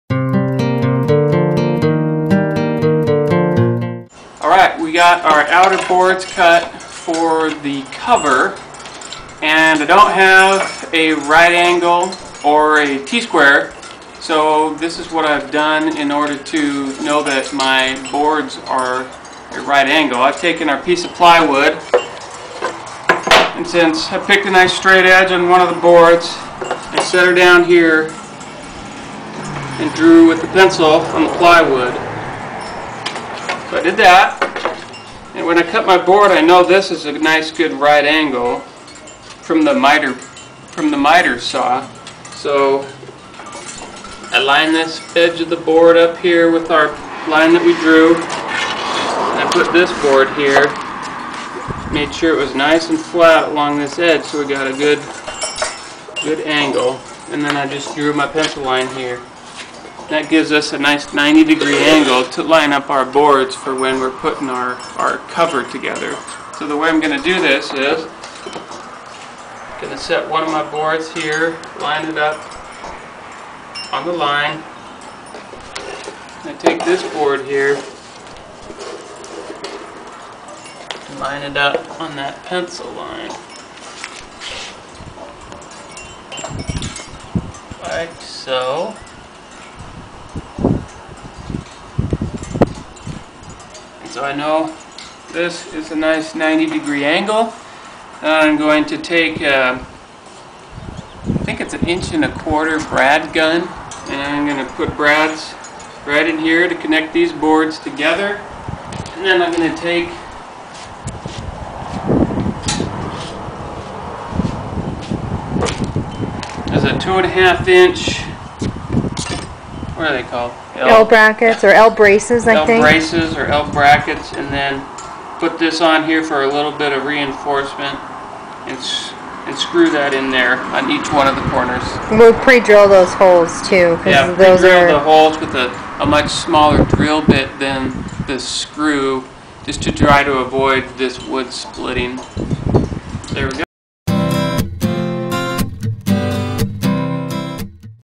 Solar Dehydrator #2
Lining up the outside "frame" boards for the cover of the solar dehydrator.